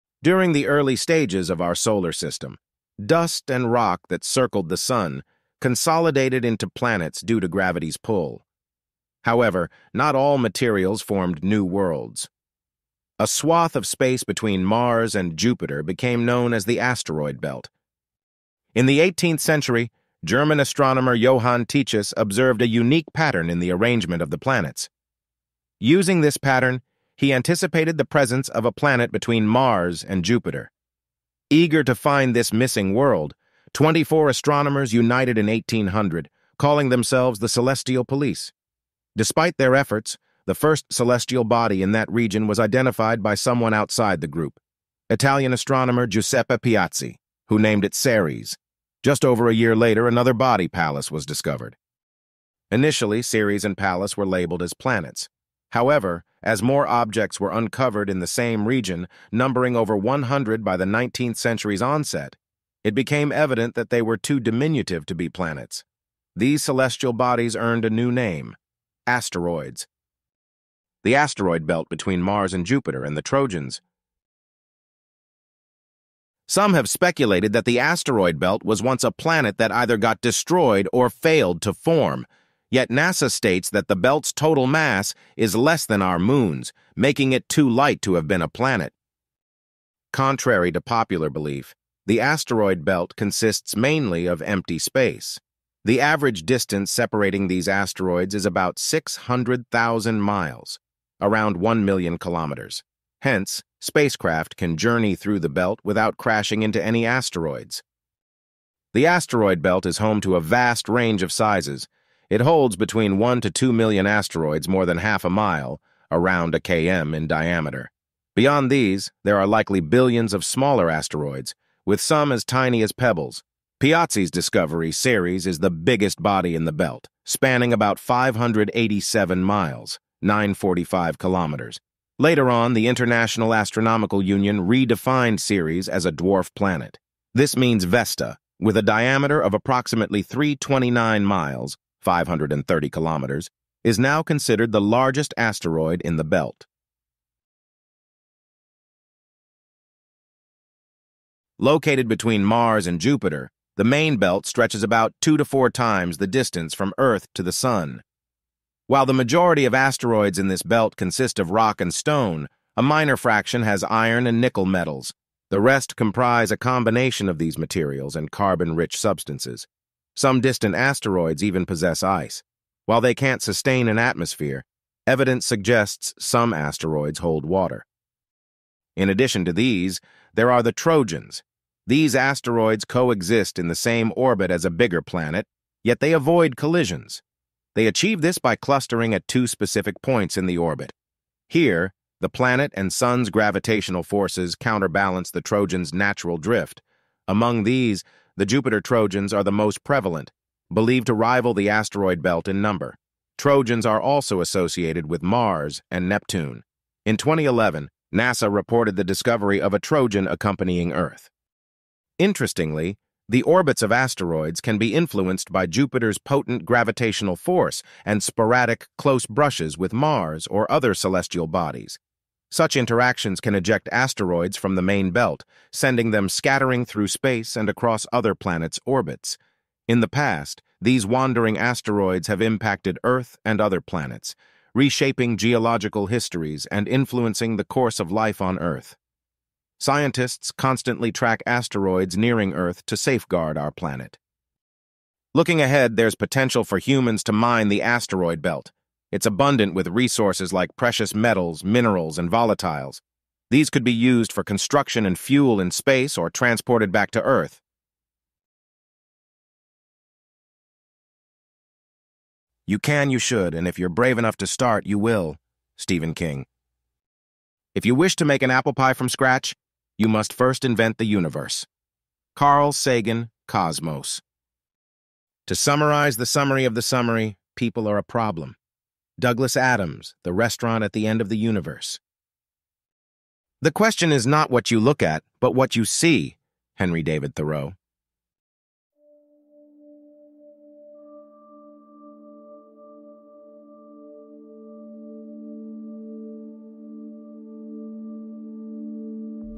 Asteroid Belt: Bridging the Gap Between Planets
Have you ever wondered what lies in the asteroid belt between Mars and Jupiter? In this video, we explore the fascinating world of asteroids and how they play a crucial role in bridging the gap between planets. Join us as we delve into the history of the asteroid belt, its composition, and the various missions that have been sent to study it. Discover how asteroids have impacted the formation of our solar system and how they continue to shape our understanding of the universe. Whether you're a space enthusiast or just curious about the mysteries of the cosmos, this video is sure to leave you in awe of the wonders of our universe.

Video creation made 10x simpler & faster with AI - Check out Fliki